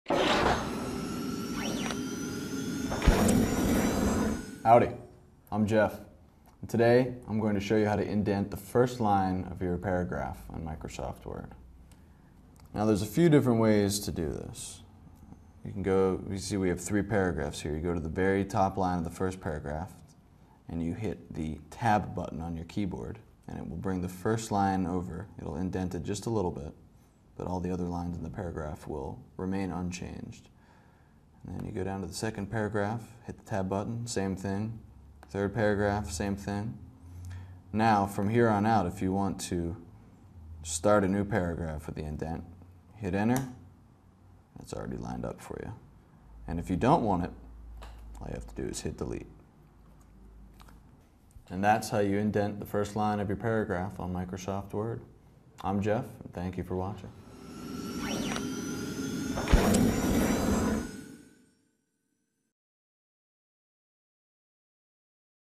How to Indent a Single Line in Microsoft Word 2010
How to Indent a Single Line in Microsoft Word 2010. Part of the series: Microsoft Word Help. Indenting a single line in Microsoft Word is something that you can do by first highlighting that element. Indent a single line in Microsoft Word with help from an experienced computer professional in this free video clip.